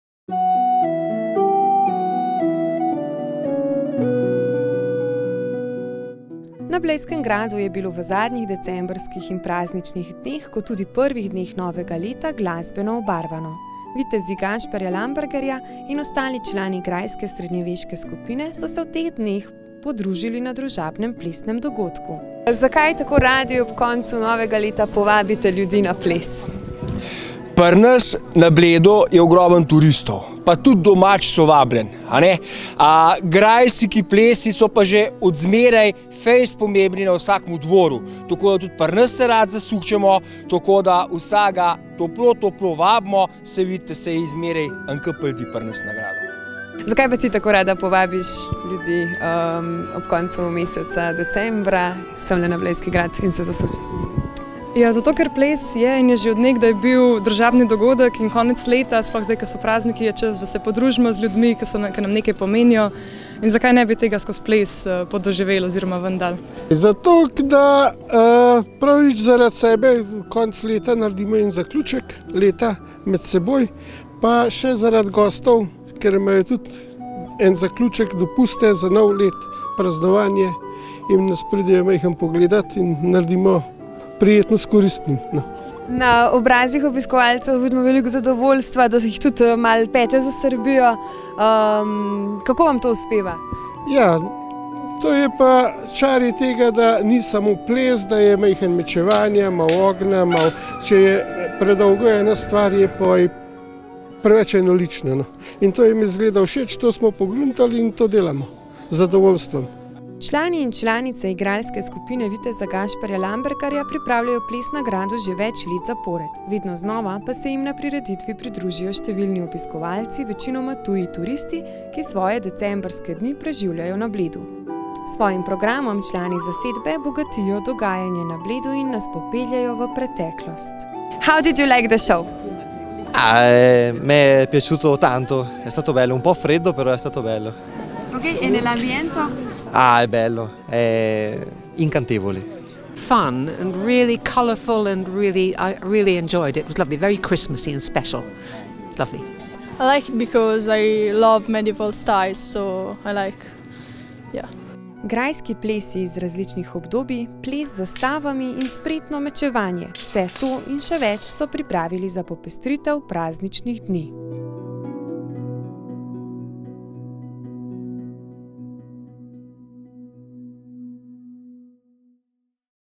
Ples z graščakom
Gledališka skupina viteza Gašperja Lambergarja šteje več kot 30 članov, med katerimi so članice in člani vseh starosti, posebnost skupine pa je v tem, da v njej delujejo celo cele družine. S svojimi doživetimi nastopi, v avtentičnih kostumih in dodelani koreografiji gledalce popeljejo med mične dvorne dame, pogumne viteze, njihove oprode. Tako je bilo tudi tokrat, na decembrskih in januarskih predstavah Ples z graščakom.

Novinarka: Sanja Despotovič
Snemalec: Žiga Resman
Montaža: Dani Tomažič